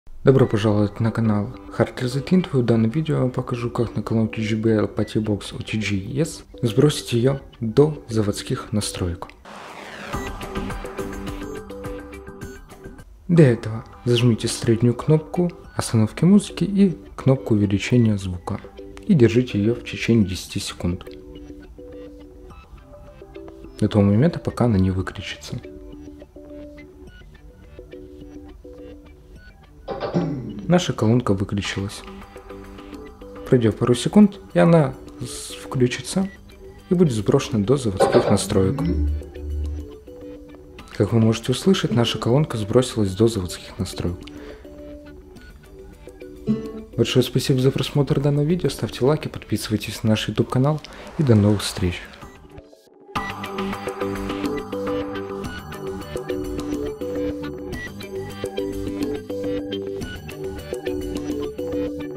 Как сбросить JBL Partybox On The Go Essential до заводских настроек
В этом видео мы покажем, как правильно сбросить колонку JBL Partybox On The Go Essential до заводских настроек. Узнайте, в каких случаях это может понадобиться, и как выполнить процесс сброса шаг за шагом. Мы также обсудим возможные проблемы, которые могут возникнуть после сброса, и как их решить. Если ваша колонка работает некорректно или вы хотите удалить все настройки, это руководство поможет вам восстановить её до исходного состояния.

В каких случаях необходимо выполнять сброс JBL Partybox On The Go Essential?
Какие шаги нужно выполнить, чтобы правильно сбросить настройки на JBL Partybox On The Go Essential?